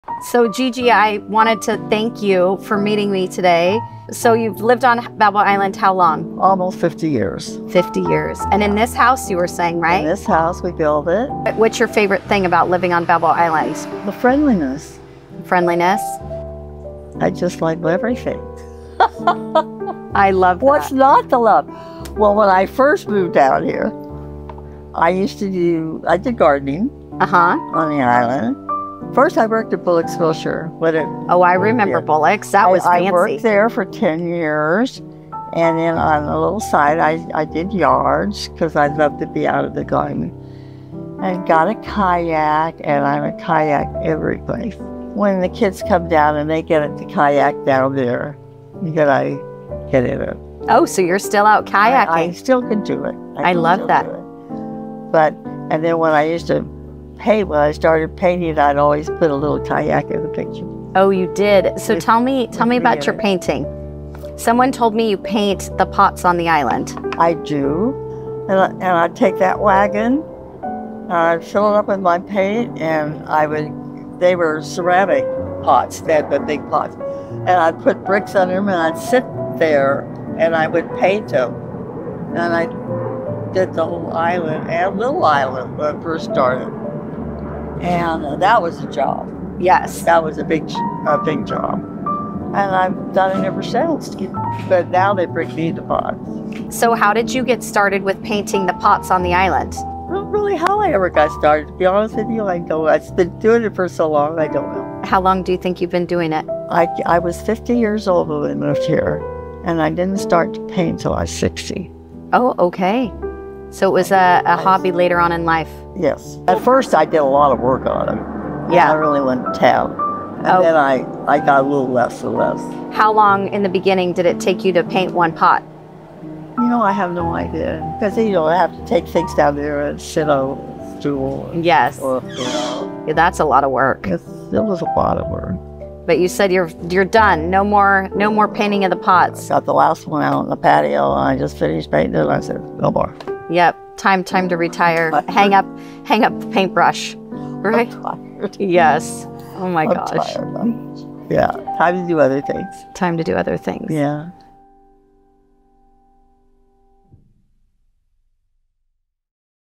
Meet GG, the painter on Balboa Island, making the community more beautiful one pot at a time.
I have some wonderful clients that told me a few months ago that I had to meet ‘GG’, my clients grandmother. I moved to Balboa Island when I was 15 and lived on Opal Avenue and came to find out that GG lived just one block away on Agate.
Although to them she goes by GG, her real name is Dottie Siemon and if you don’t know her, you might know her work.  Dottie, who took up painting when she was in her 60’s, loved to visit artist John Botz in his Laguna Beach gallery. One day he asked her why she didn’t paint. She replied, “Would you teach me?” Since he didn’t teach, he referred her to a studio in Cannery Village, where she spent a few years taking lessons.
As a way of giving back to the place she calls home, she agreed when the Balboa Island Improvement Association asked her to decorate street-end clay planter pots years ago. She has painted every planter with variations of yellow, pink, blue and green tulips above a sailboat scene.
Summing up life on Balboa Island, Siemon said: “My favorite thing about living on the island is the people and going down to the end of my street and sitting on the bench where I can look at the boats and water. It’s very peaceful.”
I wanted to spotlight Dottie, as she is a true representation of the Spirit of Balboa Island, and a reminder that you are never to old to try something new. Next time you’re walking around the island, take a look at the pots at the end of every street. When you see the beautiful paintings, you’ll think of Dottie and what a great neighbor and treasured part of the community she is.
Wishing her a happy birthday, as she just had a birthday, since I filmed this and turned 97 years old on November 2nd. Happy birthday GG!!!